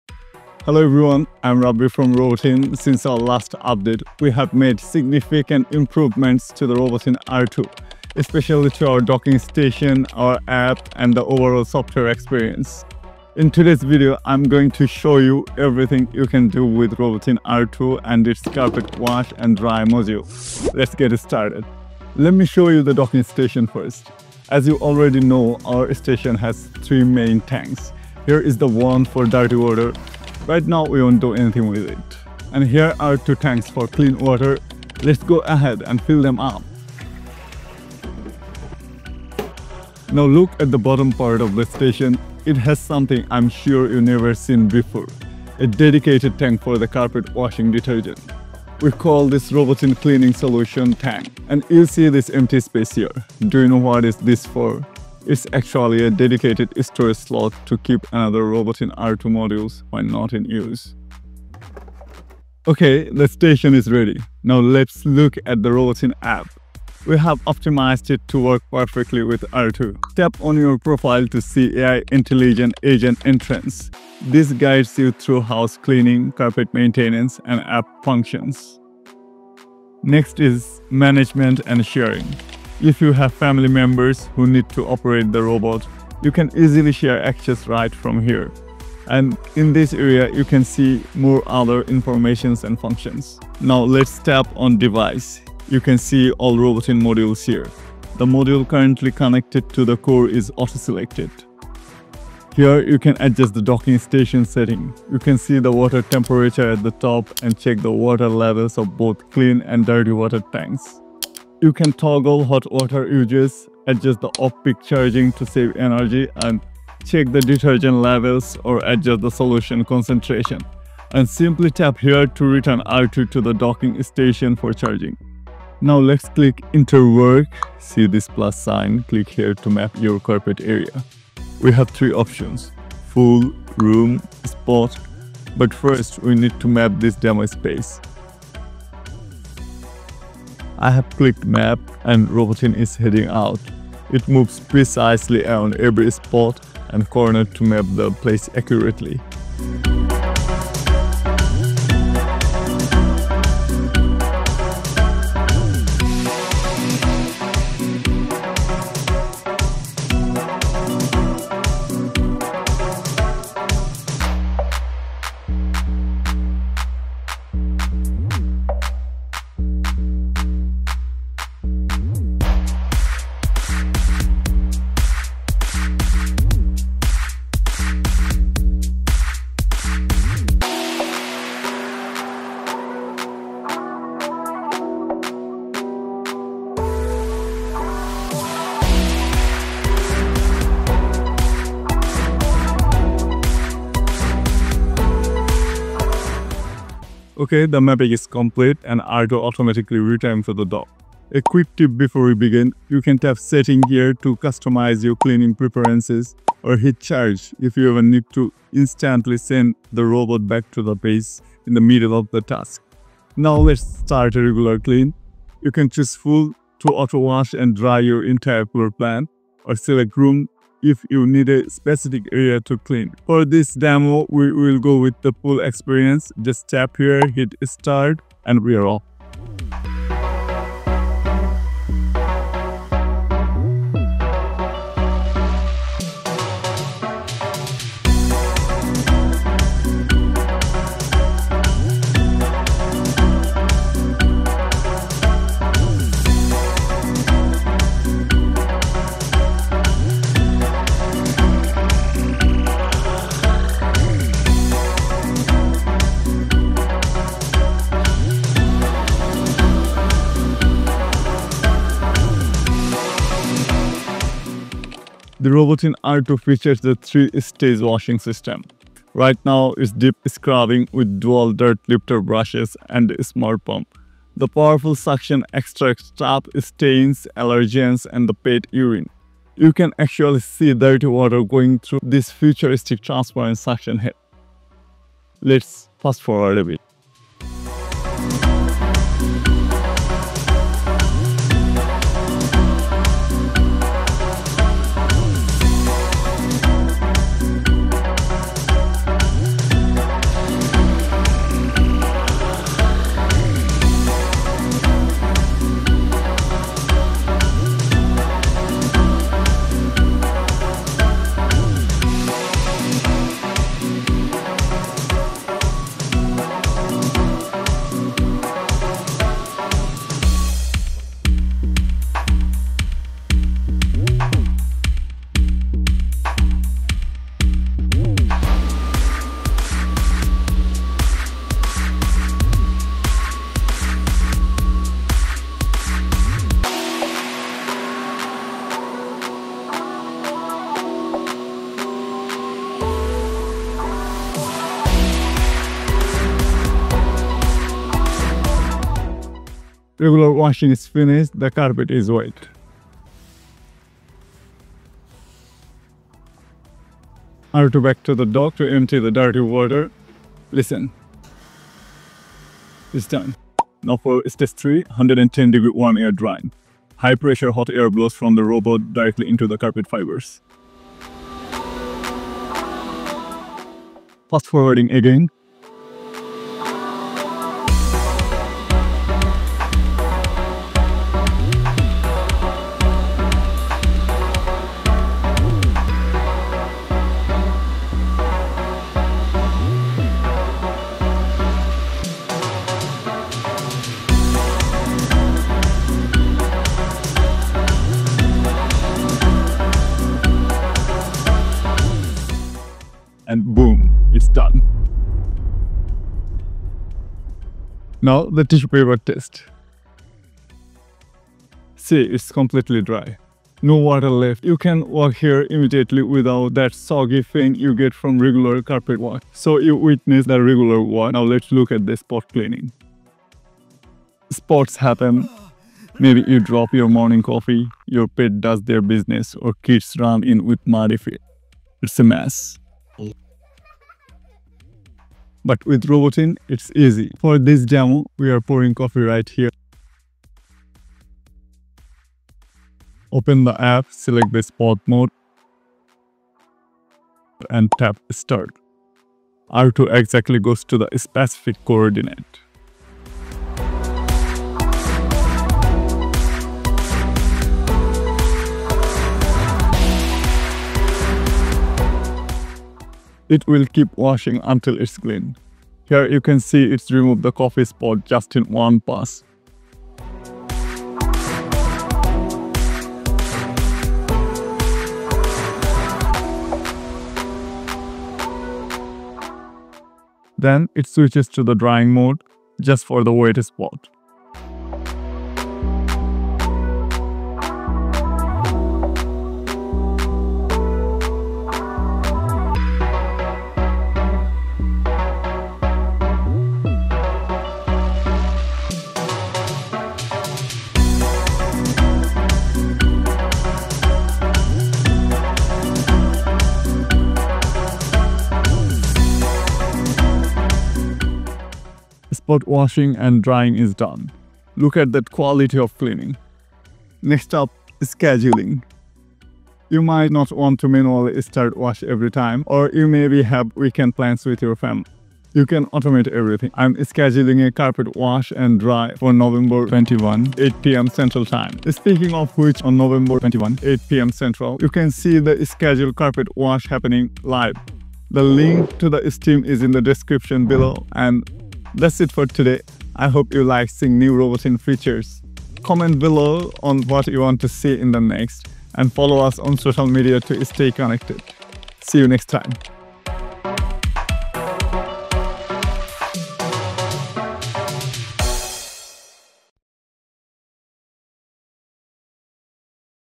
This Robot Actually Wash & DRIES Your Carpet? 🤯 Robotin R2 Full Demo & Test
Is this the future of carpet cleaning? Meet the new Robotin R2. In today's video, Rabbi takes you through the ultimate test of the Robotin R2’s Carpet Wash & Dry Module. We aren't just vacuuming; we are deep scrubbing, extracting, and most importantly, DRYING your carpets with 110°F warm air.

🔴 LIVE CARPET WASH EVENT: Join us on Nov 21 at 8 PM CT to watch the Robotin R2 clean live!

📝 IN THIS VIDEO: We walk you through the massive upgrades to the Robotin R2, including the new docking station with dedicated detergent tanks, the AI-powered App, and the 3-stage washing system. We put it to the ultimate test with a coffee spill and the "Tissue Paper Test."

⏳ TIMESTAMPS:
00:00 - Intro & What's New
00:22 - The New Docking Station Tour
00:40 - Robotin Cleaning Solution & Storage
01:03 - The New Robotin App (AI Agent & Sharing)
02:20 - Mapping the Room
03:45 - The Regular Clean Test (Full House)
04:45 - Deep Scrubbing & Transparent Suction
06:03 - Stage 3: 110° Warm Air Drying
06:47 - The Tissue Paper Dryness Test 🧻
07:01 - Spot Cleaning Test (The Coffee Spill)
08:31 - Before & After
08:36 - Scheduling & Automation
08:46 - Live Stream Announcement

🚀 KEY FEATURES OF ROBOTIN R2:

3-Stage Washing: Deep scrub, powerful extraction, and warm air drying.
110° Hot Air Drying: Leaves carpets dry to the touch instantly.
Smart Station: Auto-refill, auto-empty, and detergent mixing.
AI App Control: Custom mapping, spot cleaning, and family sharing.

👇 JOIN THE ROBOTIN COMMUNITY: Follow us for updates and cleaning tips!